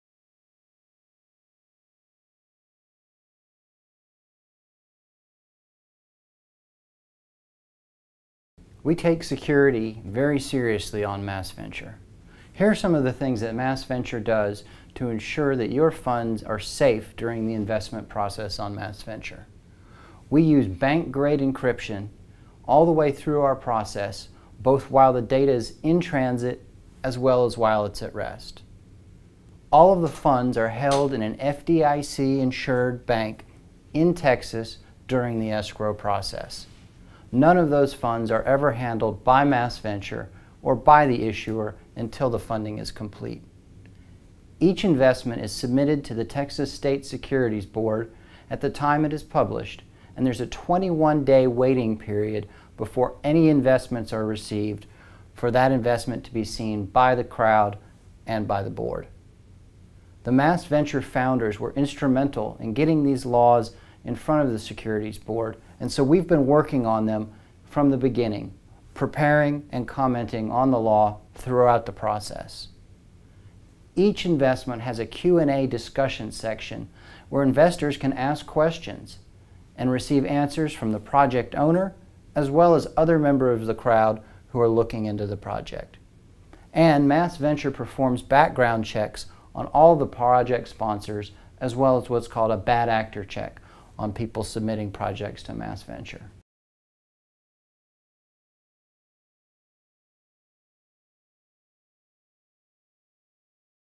How Do We Make Real Estate Equity Crowdfunding Secure
So now that you can invest in real estate projects through equity crowdfunding, you might be wondering how we make sure it's safe to do so. We work hard on making investment crowdfunding a secure way to diversify your income and make money, and in this video we'll tell you how.